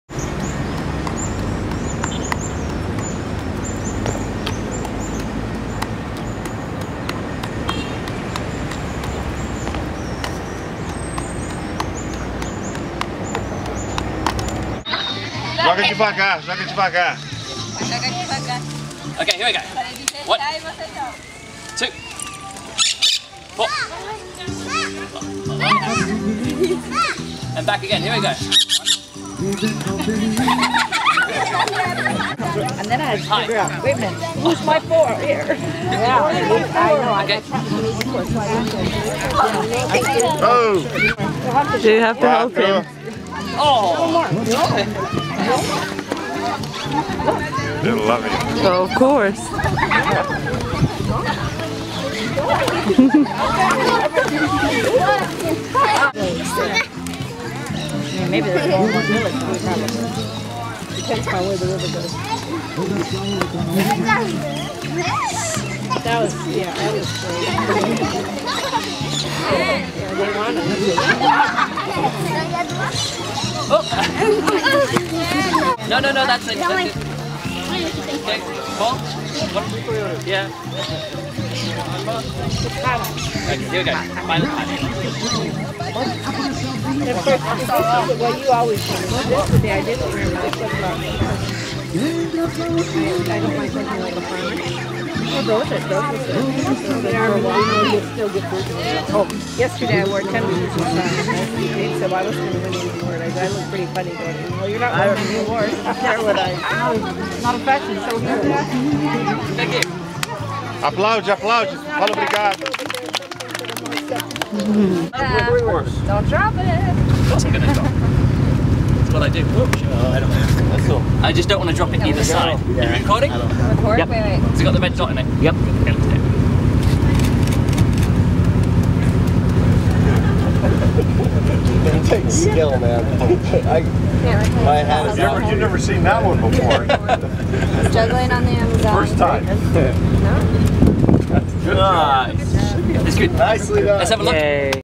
Juggling in Brazil - International Juggler 2011
Here are some clips from my latest trip, sailing up the Amazon. First, I juggled on either side of the equator, near the city of Macapa.

The middle part is from a stop at a small village, and I put on an impromptu show for all the kids, which lasted way longer than I show here. If I'd have known all the kids were there I'd have taken my diabolo and done more of a street show.

The last part is from a piranha fishing trip I took with the crew of the Prinsendam. It's not actually on the Amazon proper, but a small tributary near Santarem.